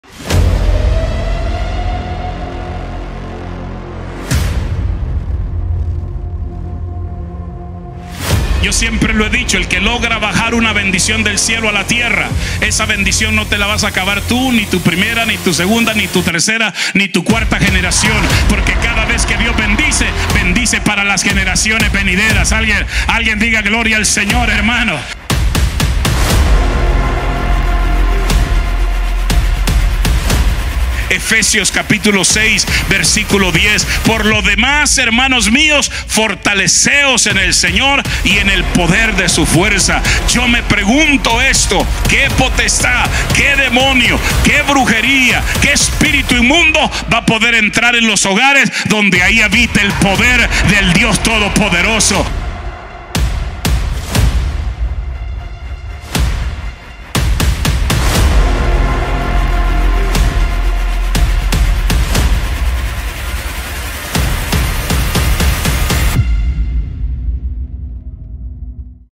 Códigos de sabiduría - Pastor Freddie Aragón
Le compartimos un poco de lo que será nuestro fin de semana del 20 al 22 de octubre de 2023. Estará con nosotros desde Tucson Arizona trayendo un palabra que incrementará nuestra fe, ven y se parte